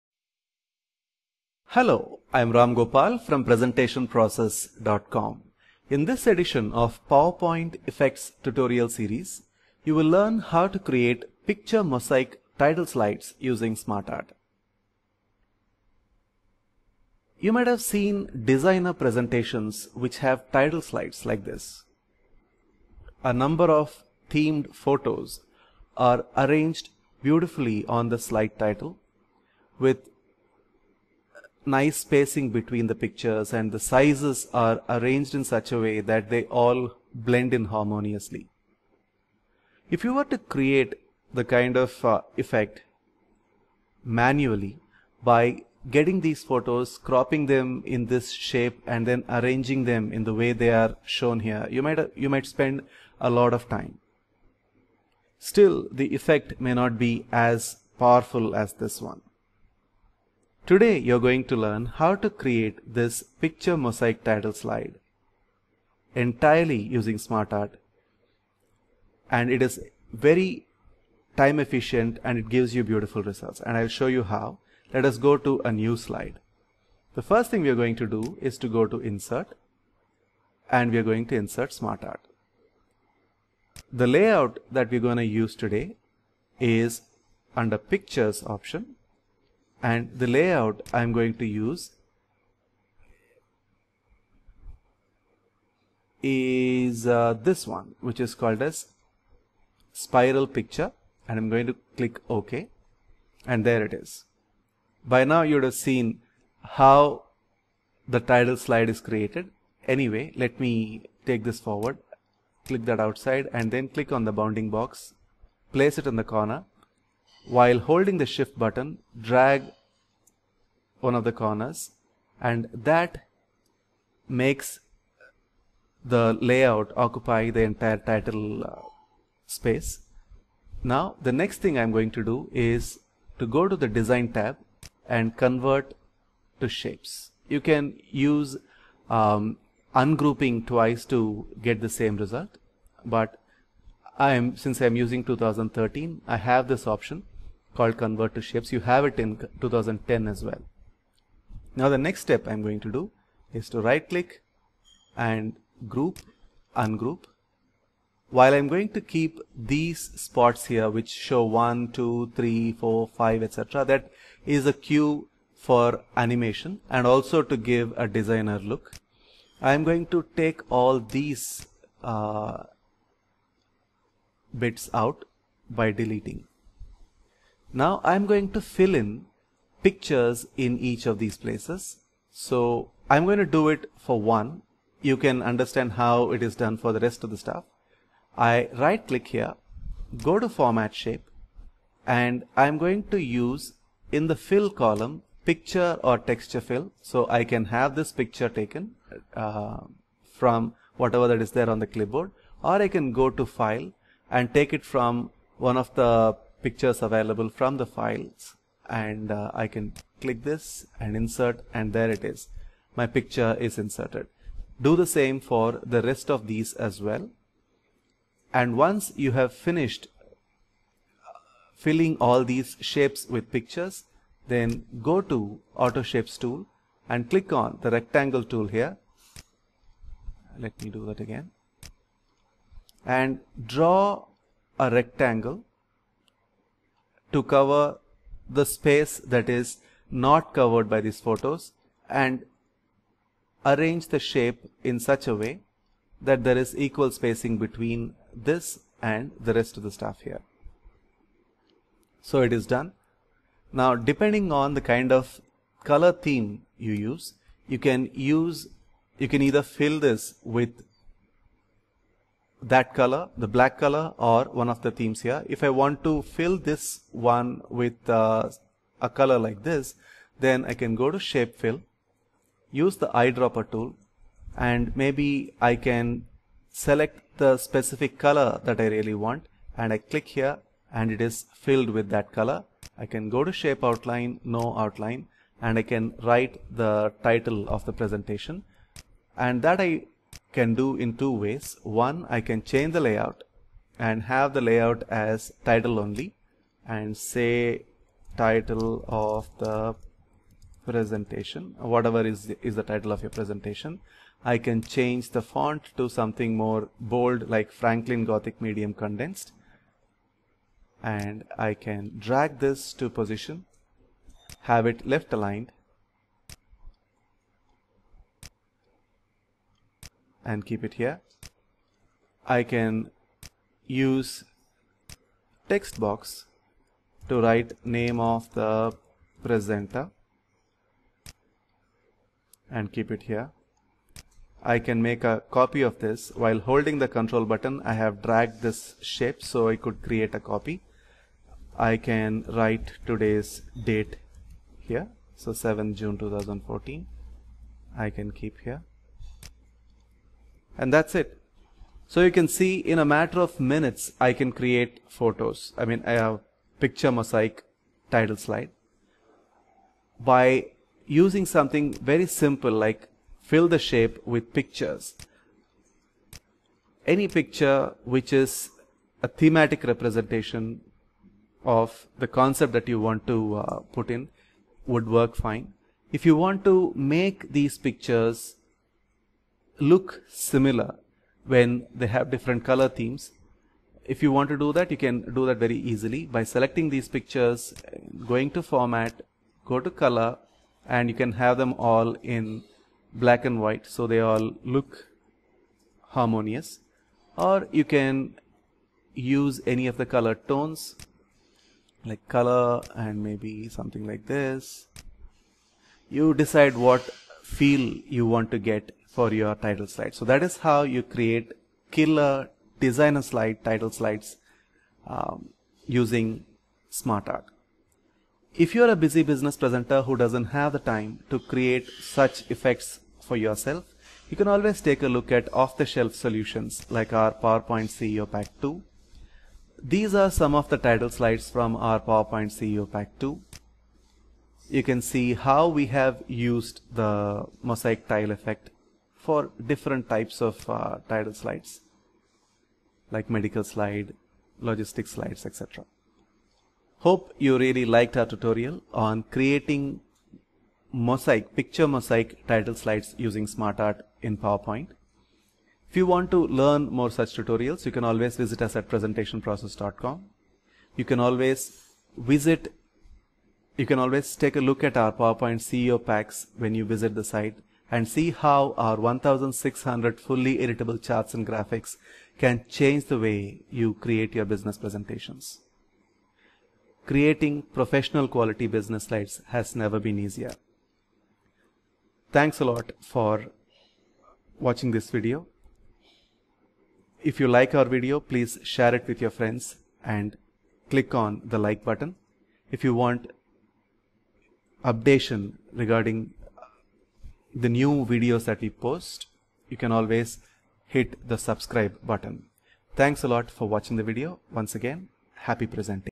How to Create Picture Mosaic Title Slides Using SmartArt: Effect Tutorial Series
Learn how to create Picture Mosaic Title Slides easily and quickly Using SmartArt. Smartart has some interesting features and templates that can be used creatively to reduce time for creating effects in your presentations.

Here is the link to 2 CEO Bundle for PowerPoint showcased in this video